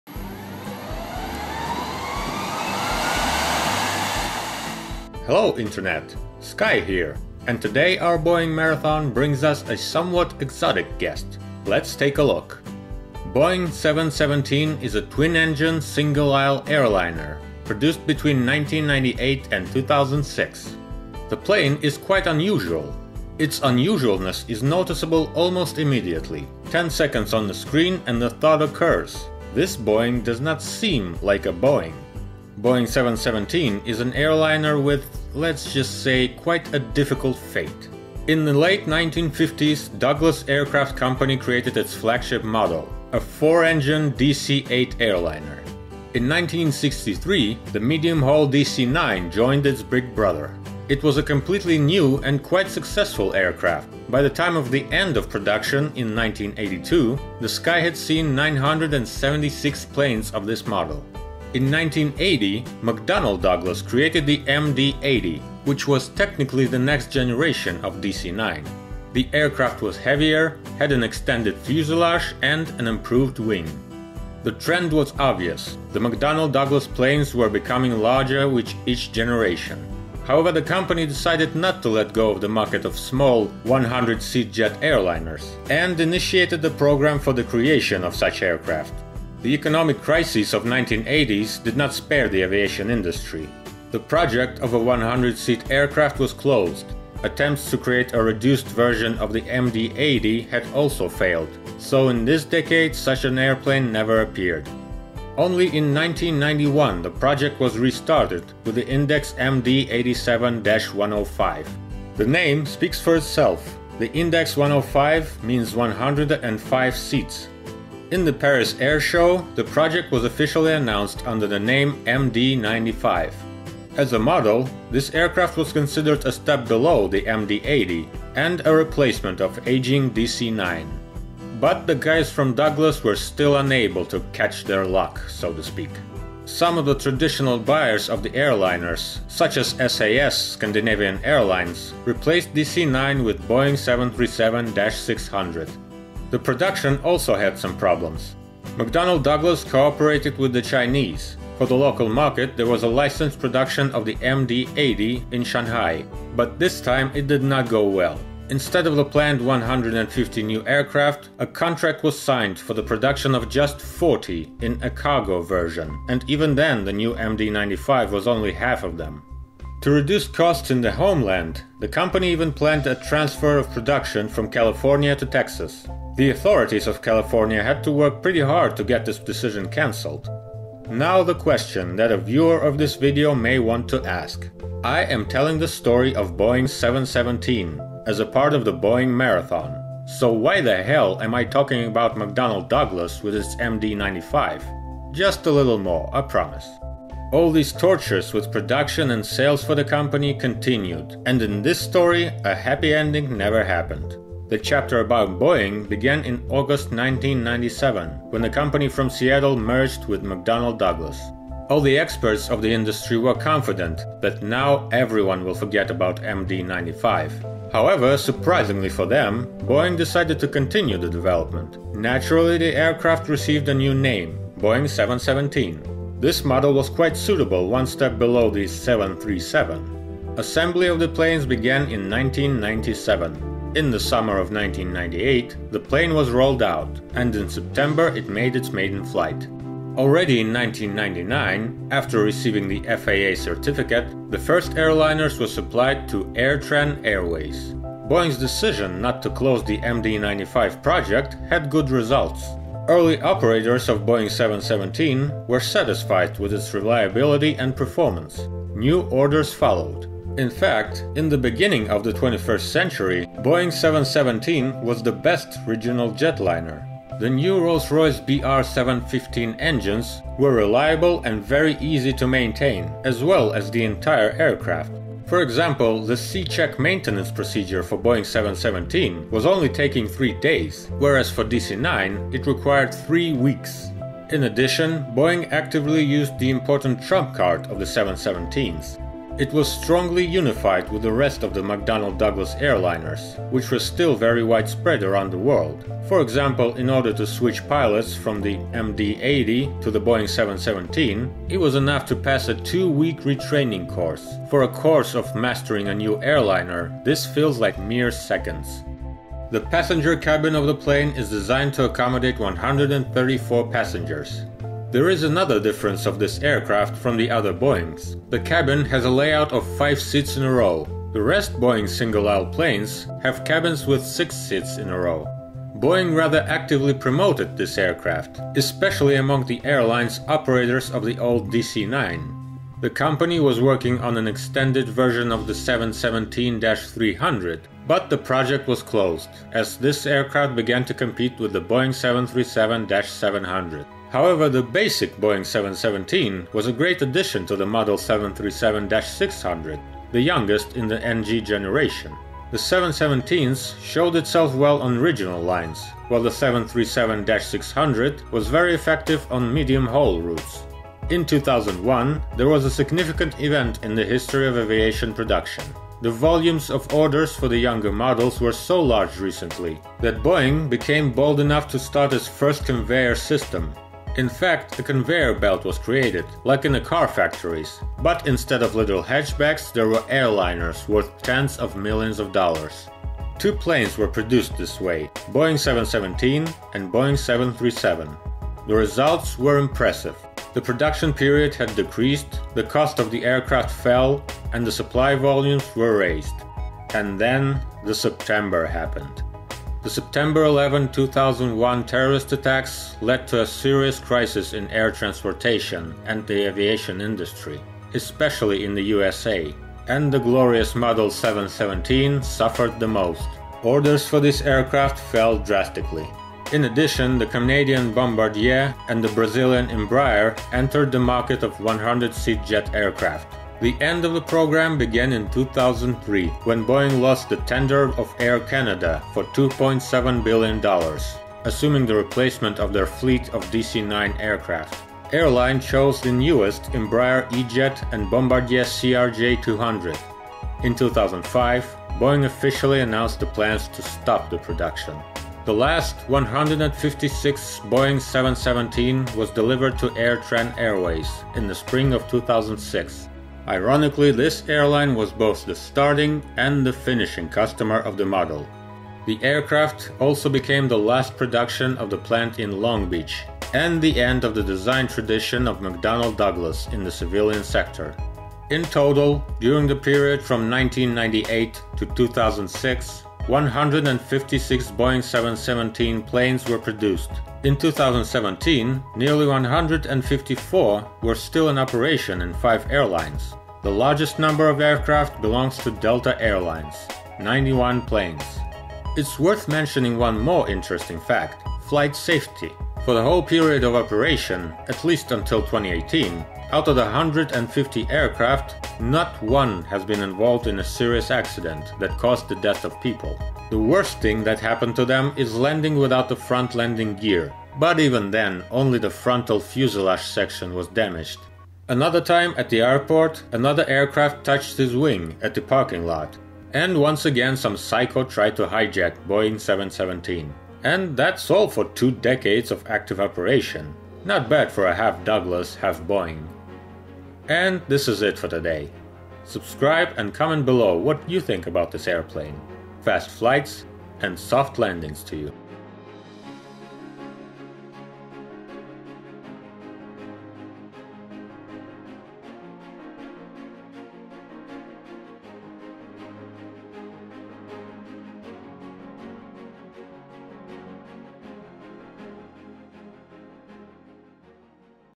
Boeing 717 - Boeing model and legacy of Douglas
Boeing 717 is a twin-engine single-aisle airliner, produced between 1998 and 2006. The plane is quite unusual.
Initially, It was developed by the McDonnell Douglas, but after the company merged with the Boeing the project was realized as the Boeing 717.